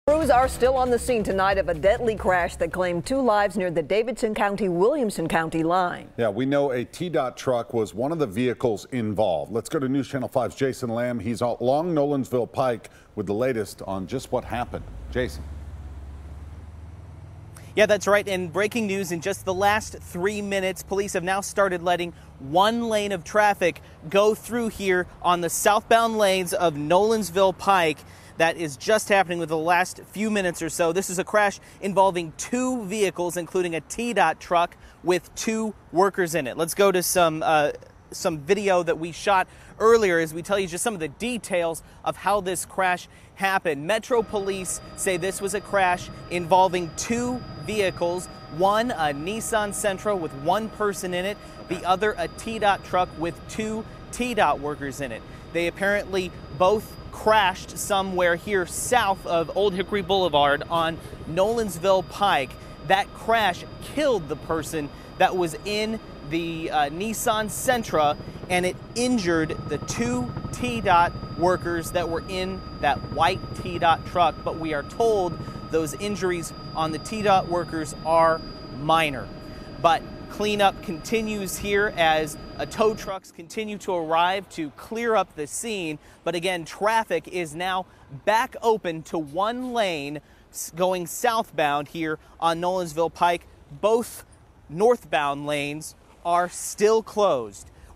Car, TDOT Truck Involved In Head-On Fatal Crash
At least one person has died in a crash on Nolensville Pike involving a Tennessee Department of Transportation truck.